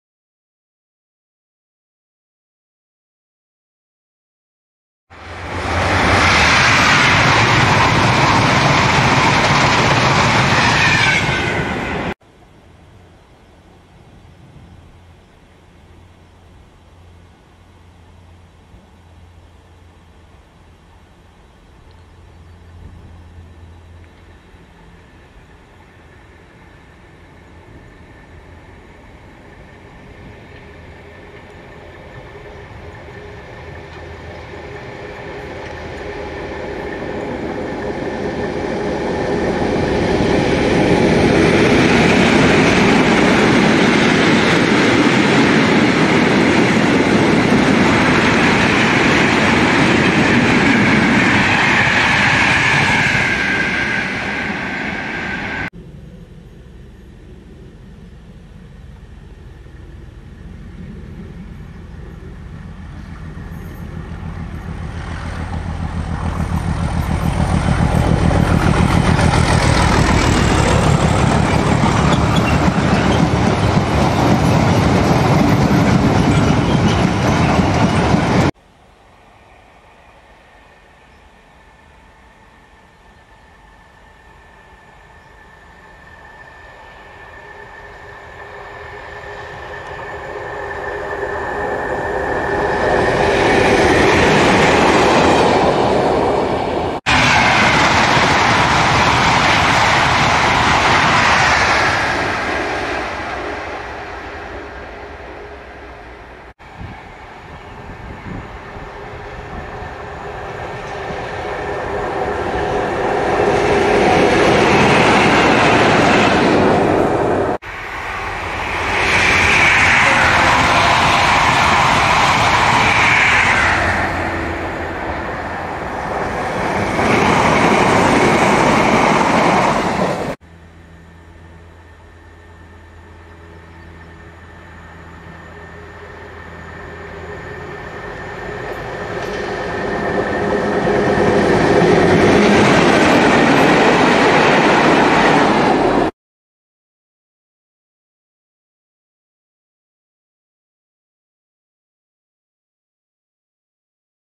Trains at Ashton near roade jct West coast mainline on a very hot 25th July 1996
A short film shot at ashton nr roade jct WCML in july 1996